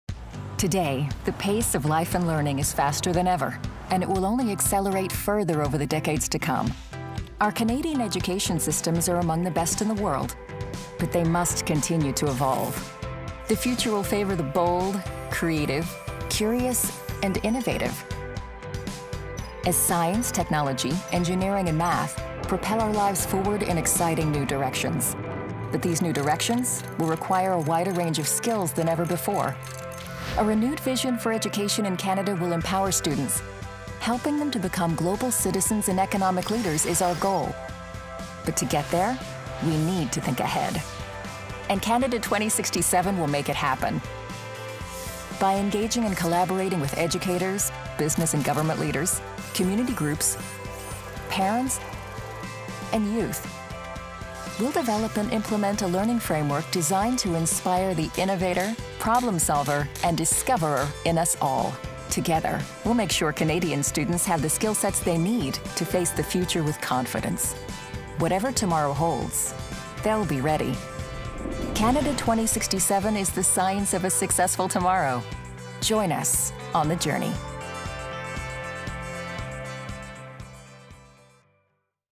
Canada 2067: The science of a successful tomorrow
Canada 2067 is engaging Canadians to collaboratively develop and implement an innovative framework for learning for the next 50 years. Working together we can inspire and nurture the innovators, entrepreneurs, discoverers and citizens we need to power Canada’s future.